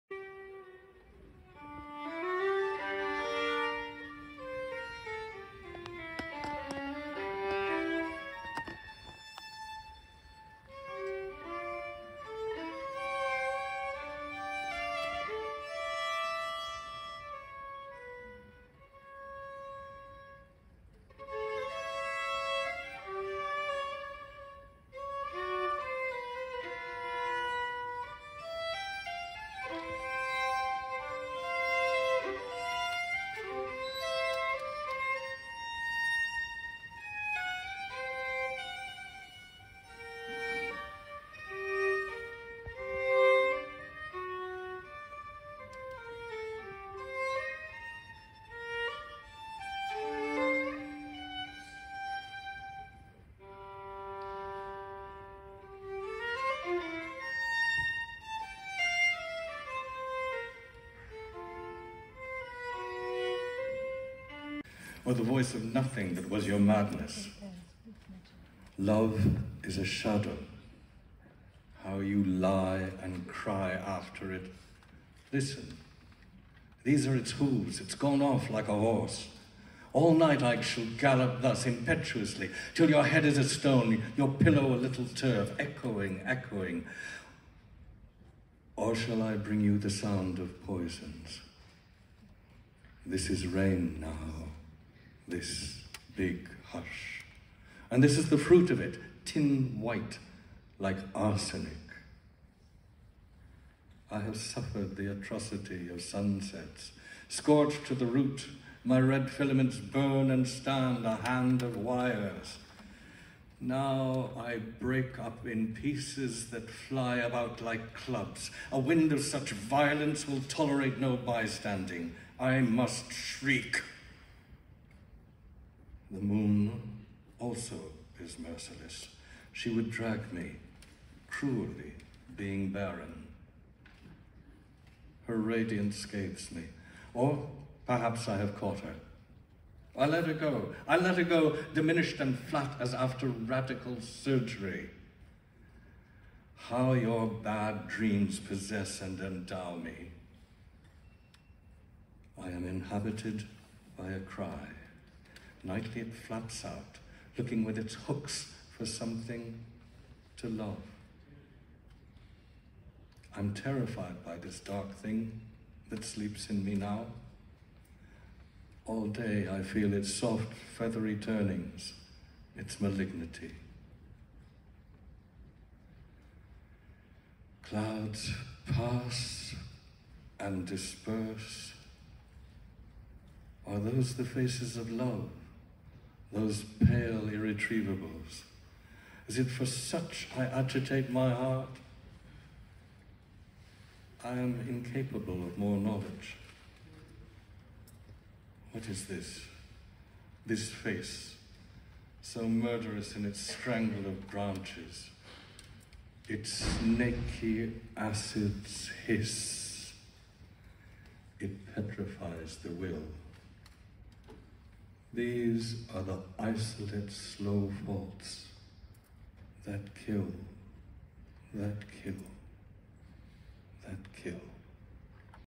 Saudha Taste of Twilight | Classical Music | Johann Sebastian Bach | Sylvia Plath | Spoken-words
A Greek-born Western Classical Violinist Dimitra Ananiadou is playing from johannsebastianbach Jon Erik Schelander is reading from SilviaPlath and bharatnatyam dancer Srimati Ragasudha Vinjamuri is rendering visual interpretation of the verses at Saudha's TasteOfTwilight directed T M Ahmed Kaysher as part of NWAMI's cultural integration event at Welsh Parliament.